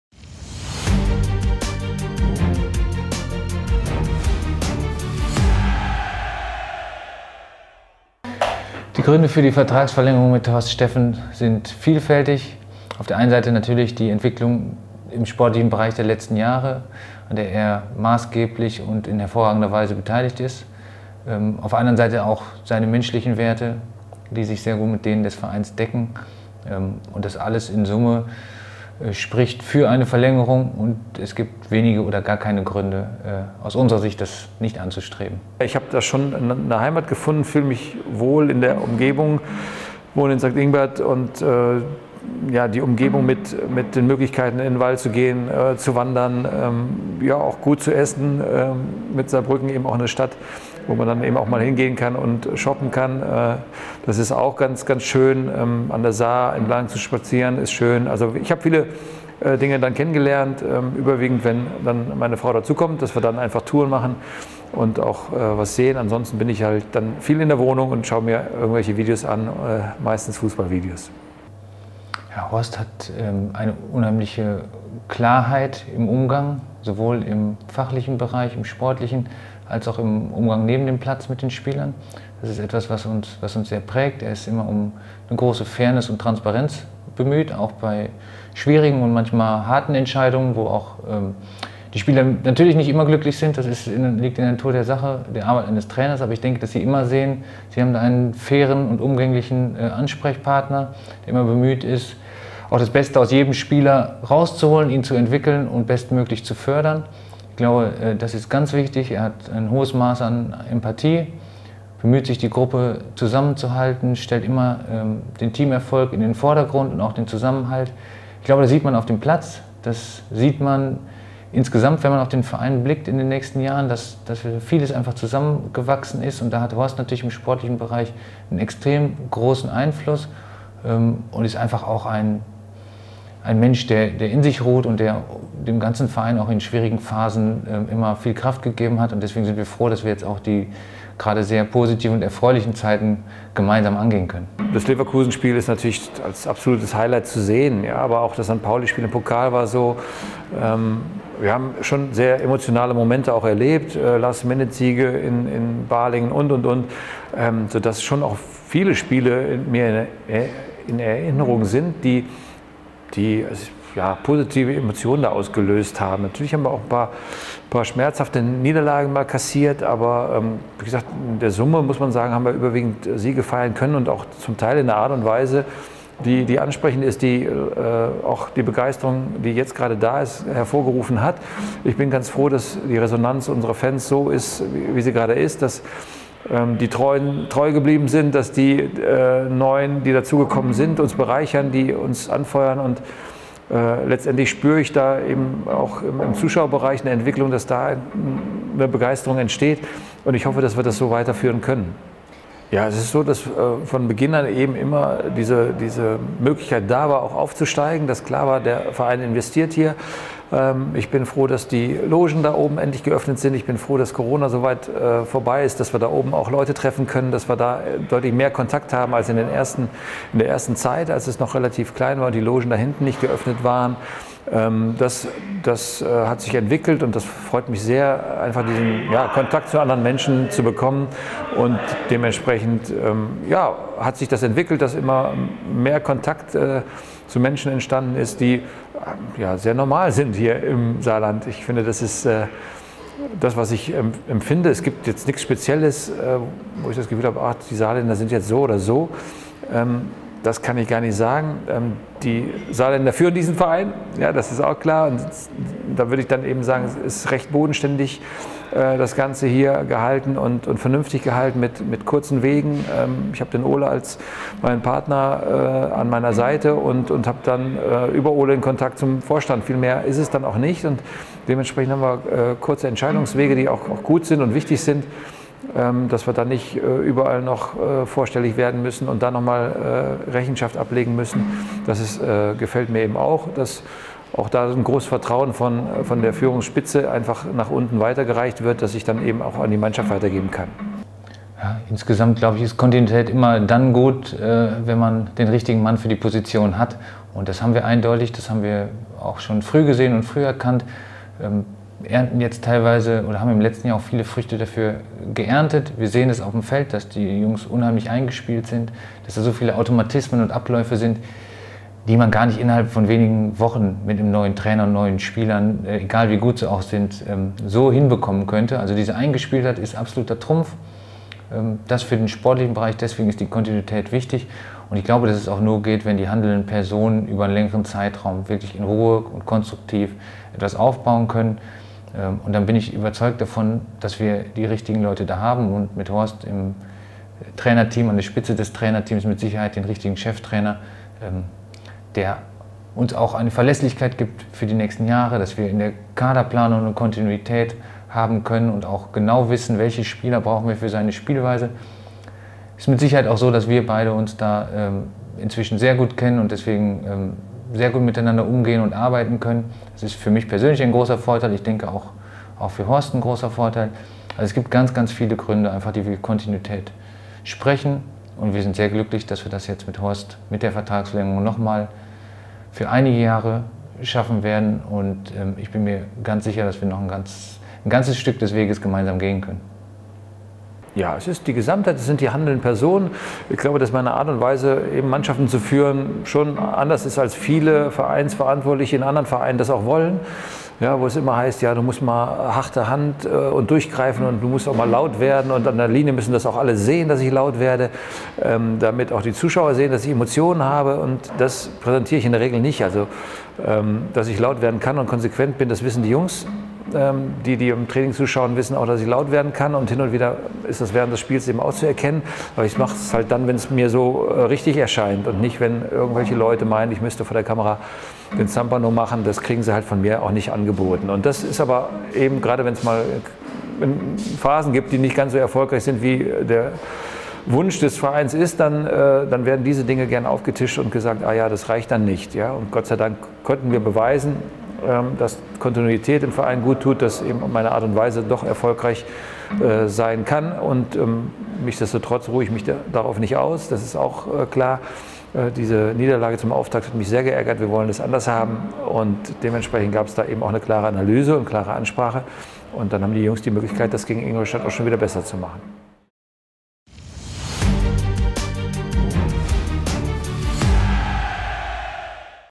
TV Elv // Inside - Vertragsverlängerung Horst Steffen
Horst Steffen verlängert seinen Vertrag bei der SV Elversberg vorzeitig bis 2026. Dazu haben wir uns mit unserem Coach und Sport-Vorstand Ole Book unterhalten.